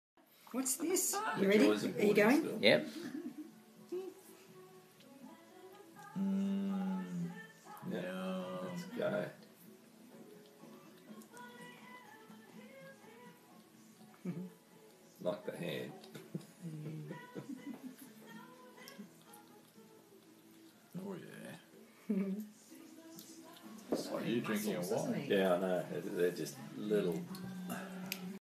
Jack the wombat bottle fed like baby wrapped in blanket
Baby wombat Jack was orphaned and badly injured in a car accident but is recovering well and being cared for by ACT Wildlife in Canberra, Australia.